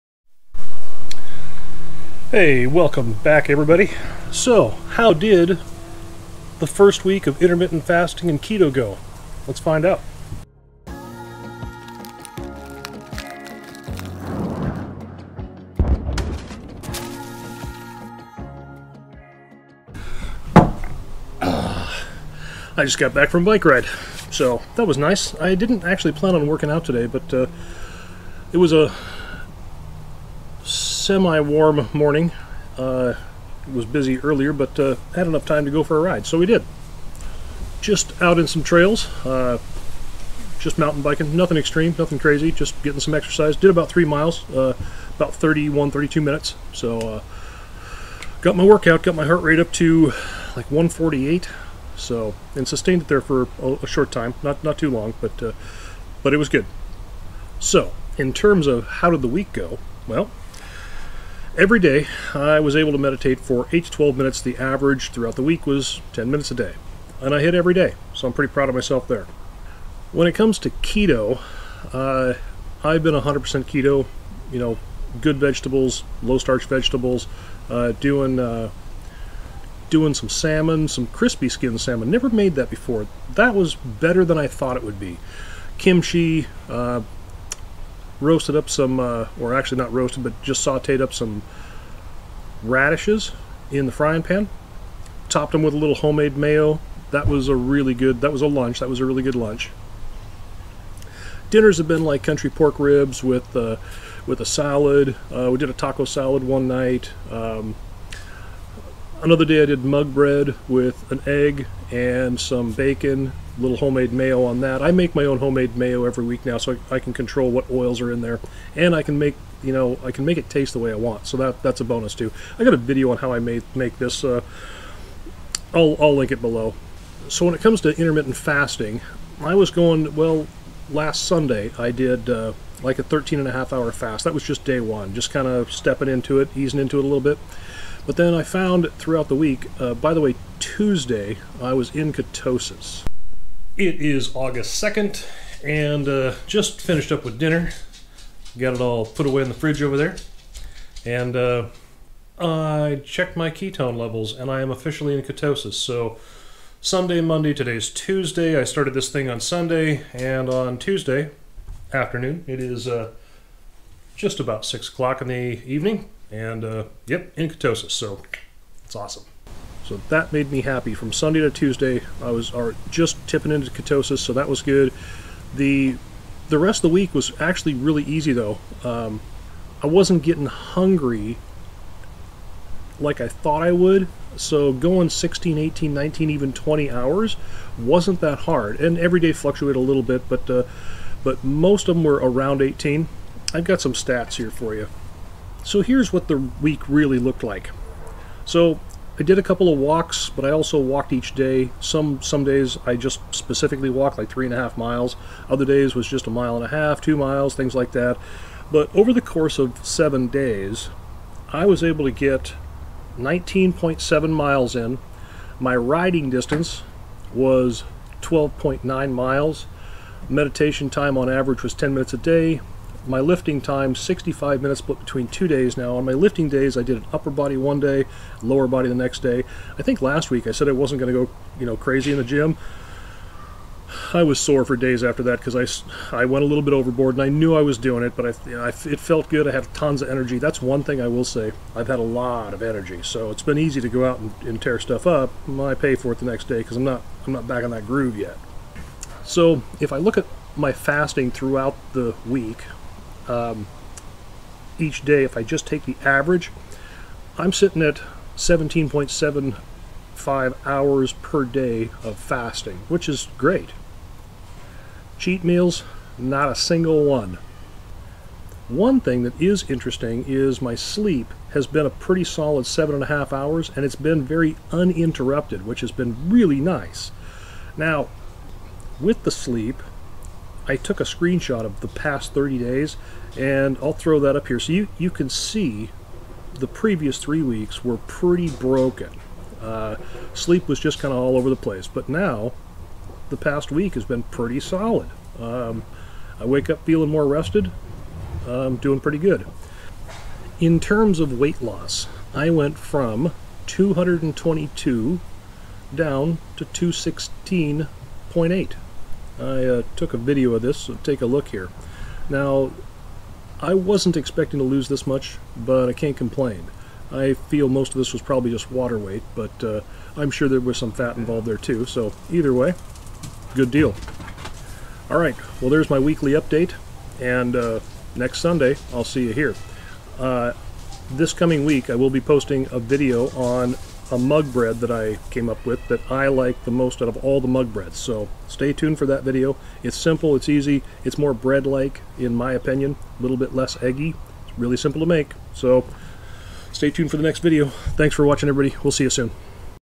Week 1, 7 day, Intermittent Fasting and Keto, how did it go?
One week of Intermittent Fasting along with Keto. I actually felt amazing, I had a ton of energy and I wasn't really hungry at all during my fasting window. Toward 20 hours of fasting I did start to get a little hungry but I could have gone longer for sure! I did daily meditation for around 10 minutes a day, I plan on bumping that up to 15 in the coming weeks. This wasn't as hard a my brain told me it was going to be.